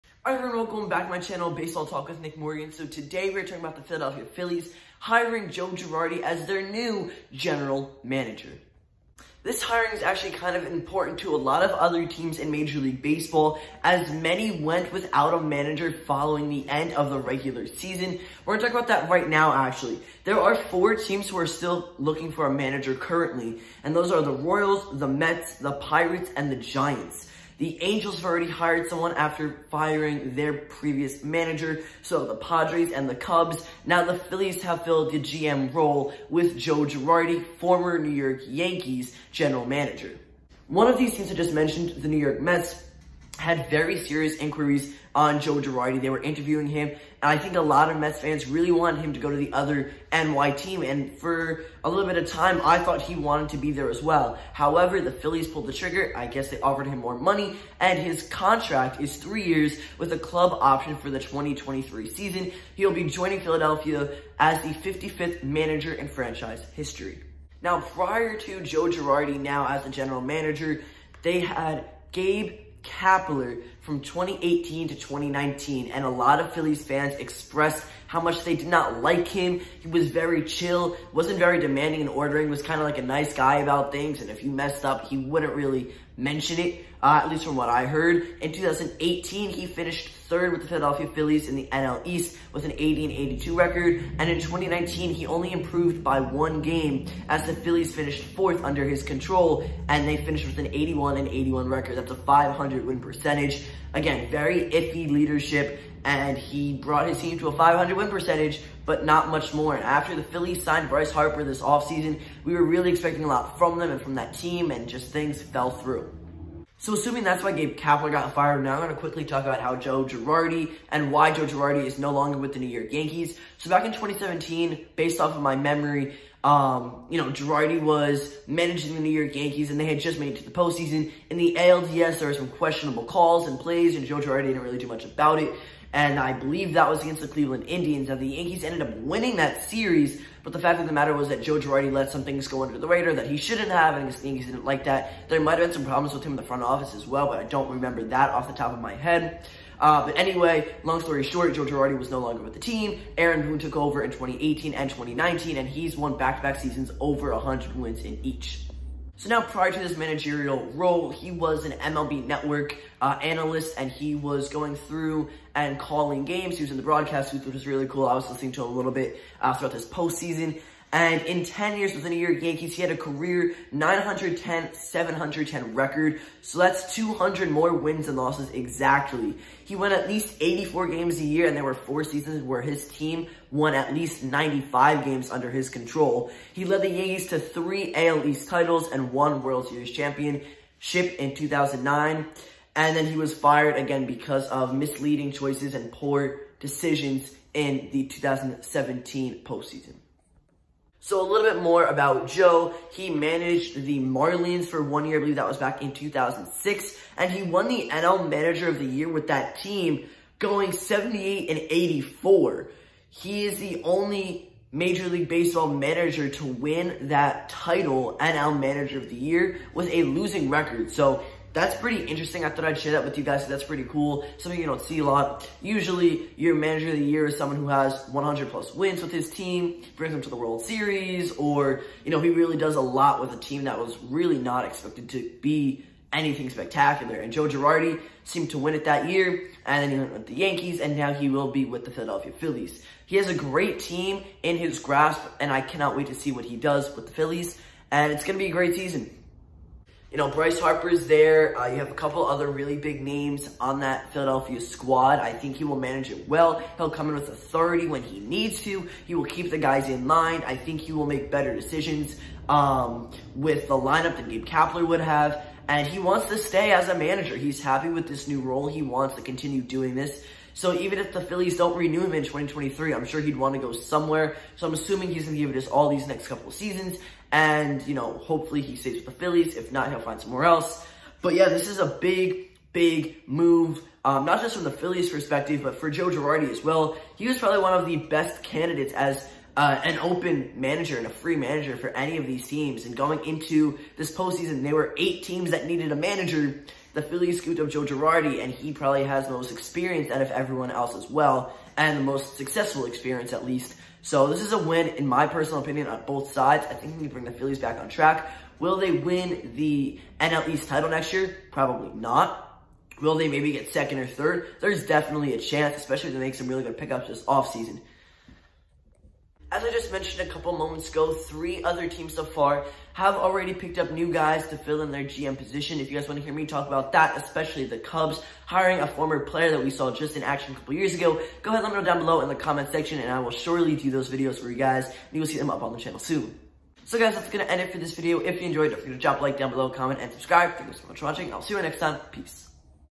The Phillies Hire Joe Girardi as Their New Manager
*Thumbnail typo! Sorry guys 😂